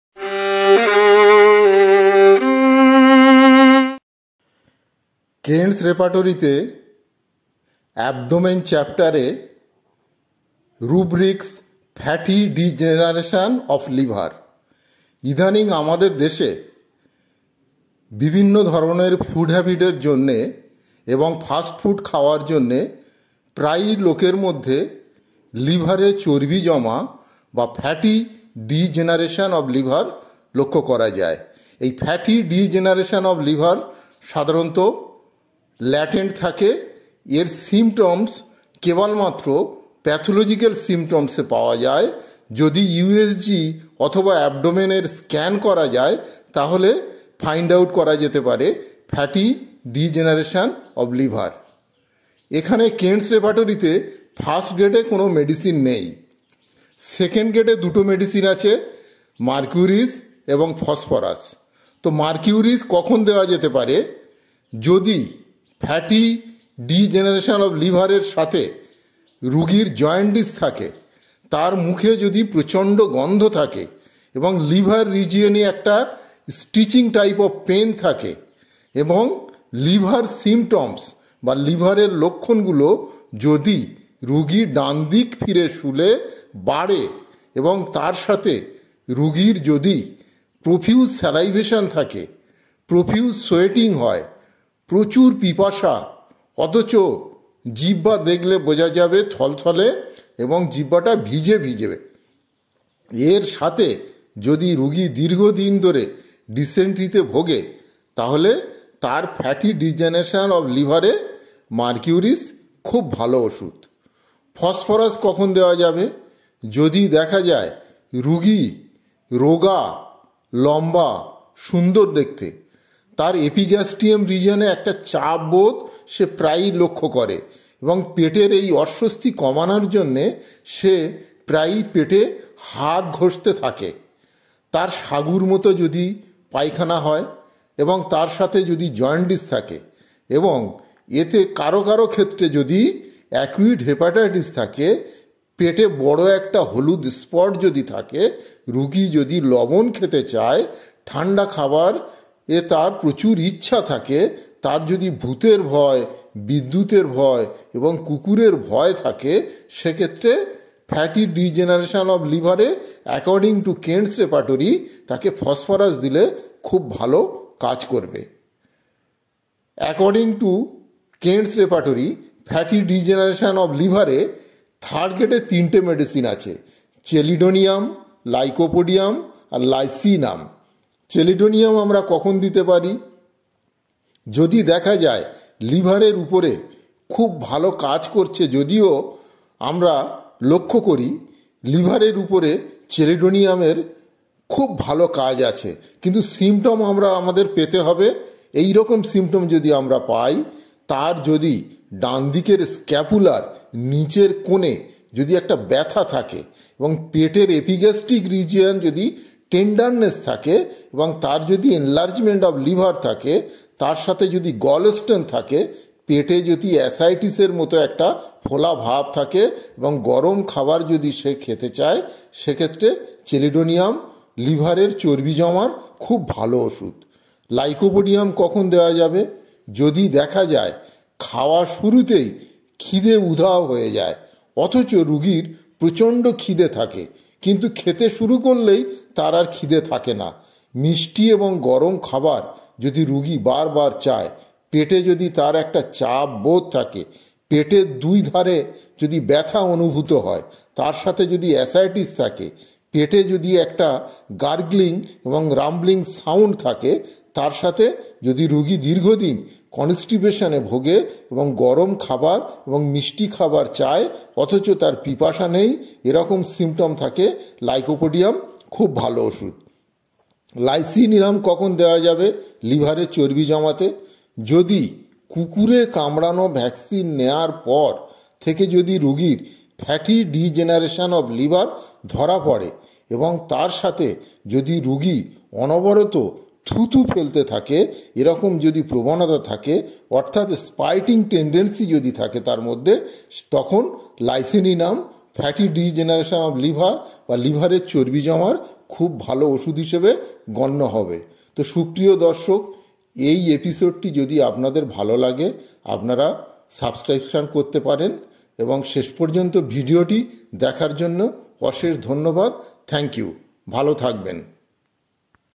Fatty Liver/Homeopathy Medicine in Kent's Repertory.
লিভারে চর্বি জমার হোমিওপ্যাথি ঔষধ কেন্ট রেপারটরি অনুসারে আলোচনায় ডাঃ আনোয়ারুল হক